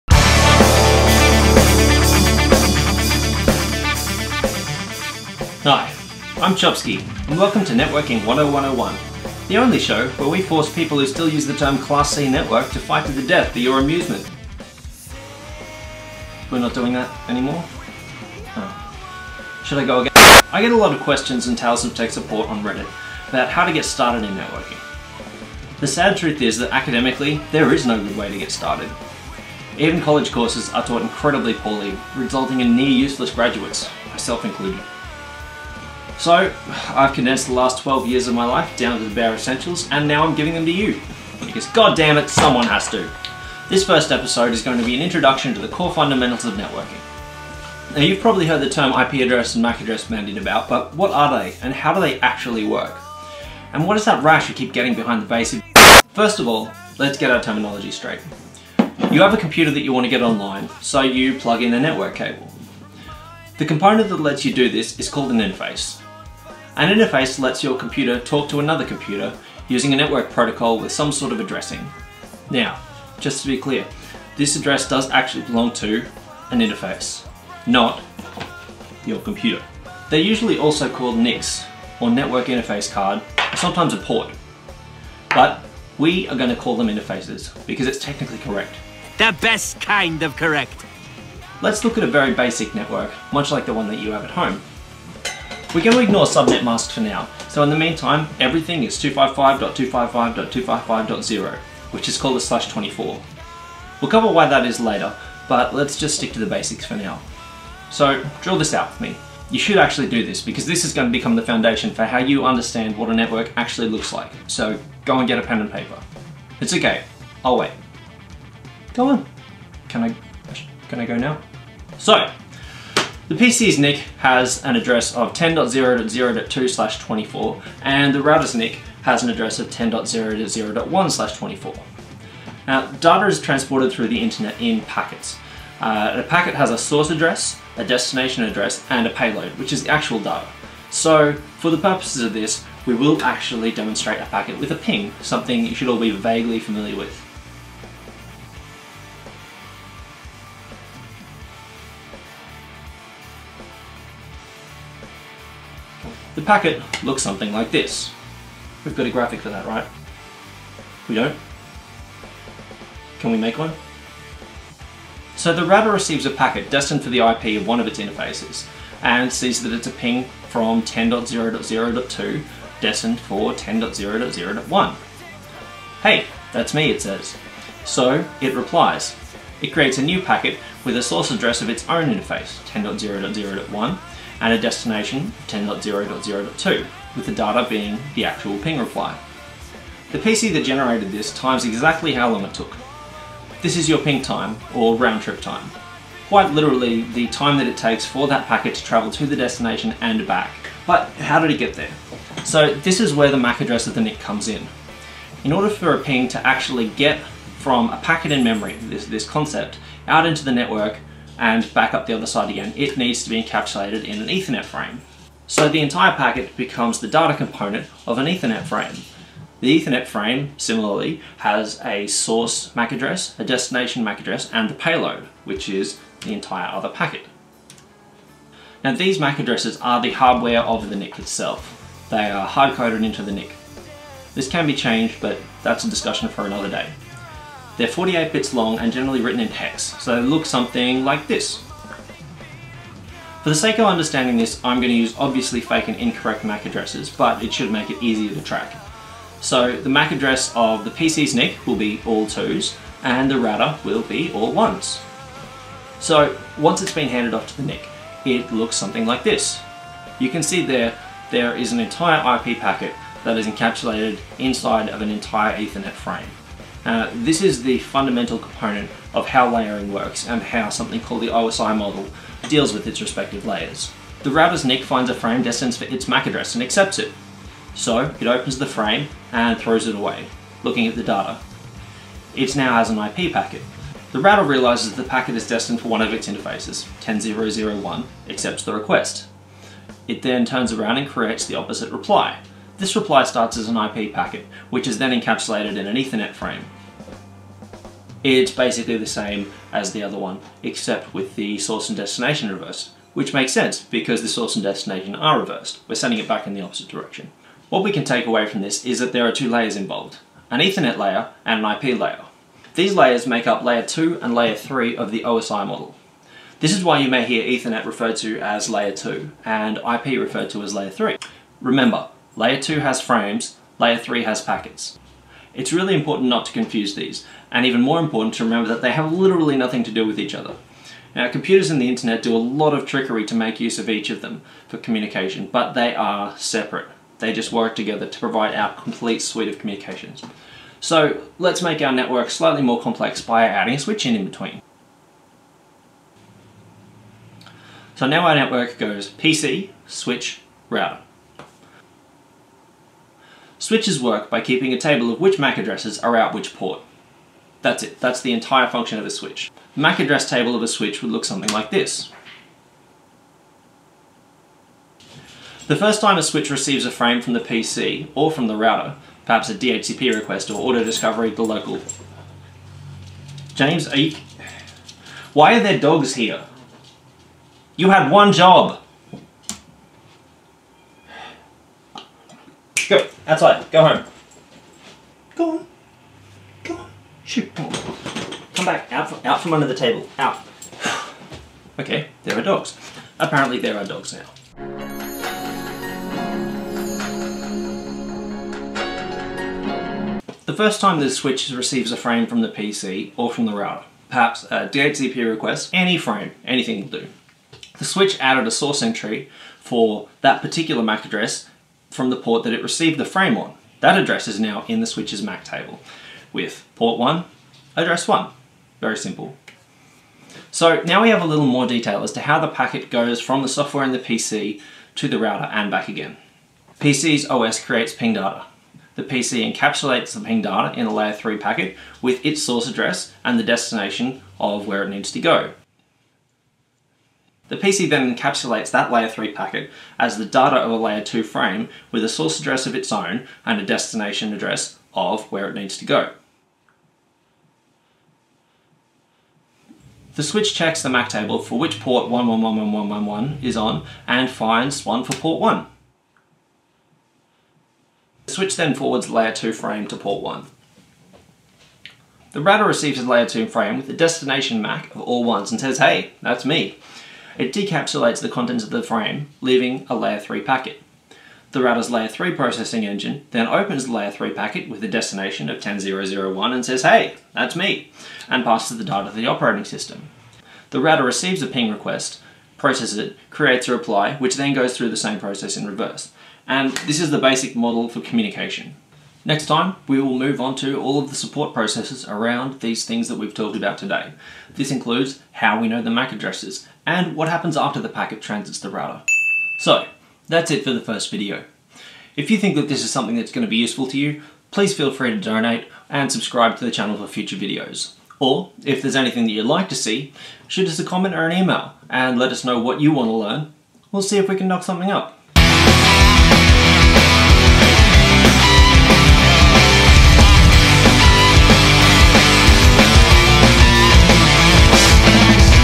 Networking 10101: Fundamental Local LAN
This episode covers basic Layer 2 and Layer 3 operation within the local LAN, and goes through what exactly happens in your network when two devices try to ping each other.